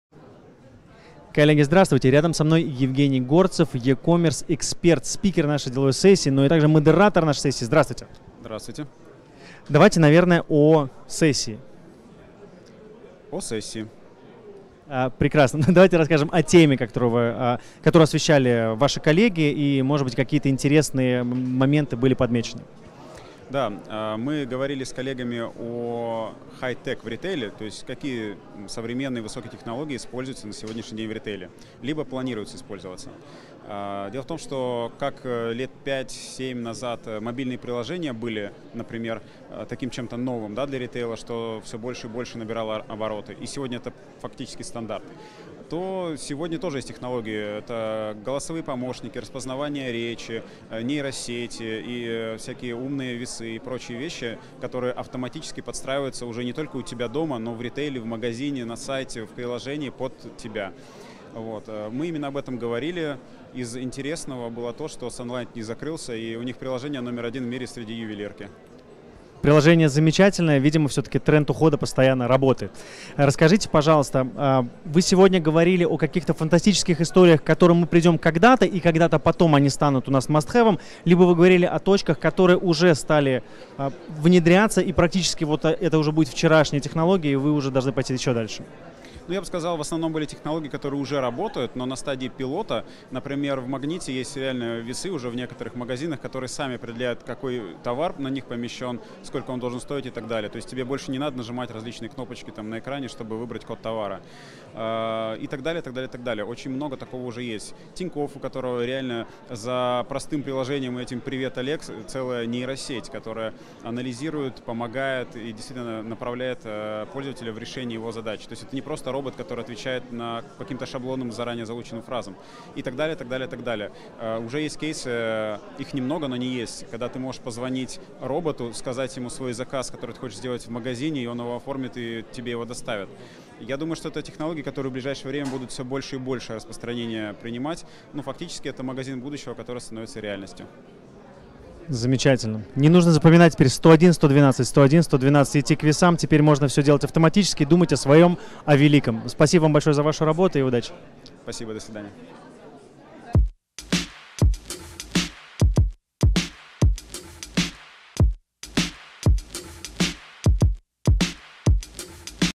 Евгений Горцев - Е-commerce эксперт на
Евгений Горцев, Е-commerce эксперт, о работе роботов и голосовых помощниках как магазине будущего